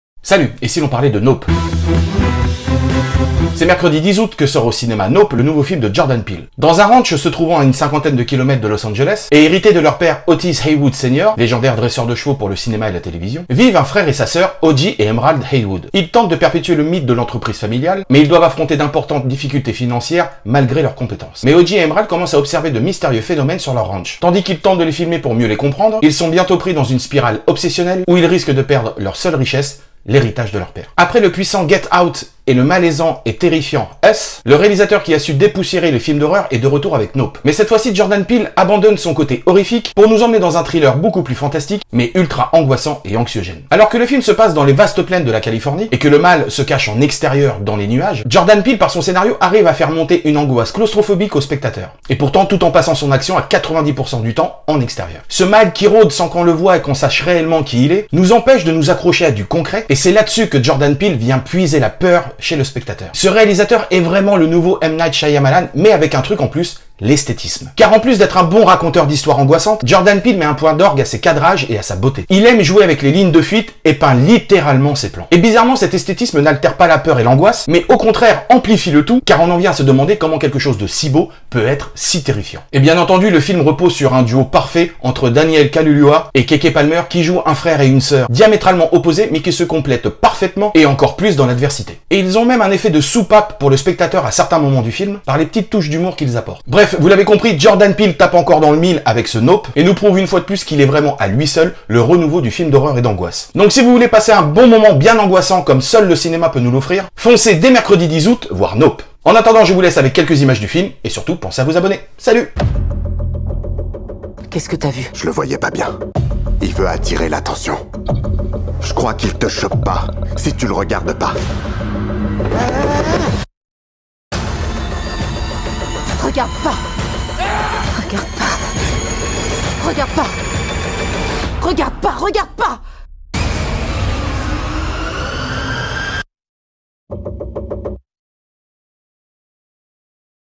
Et si l'on parlait de... (short version) NOPE de Jordan Peele
Après l'horreur avec GetOut" et US , le talentueux Jordan Peele passe au fantastique avec une main de maître en nous proposant Nope !
Porté par son acteur fétiche DanielKaluuya et la talentueuse KekePalmer, ce film va réveiller en vous des angoisses terrifiantes ! Je vous dis tout et sans spoilers dans ce nouvel épisode de "Et si l'on parlait de..." consacré à "Nope". En salles le 10 août  ! Enjoy !!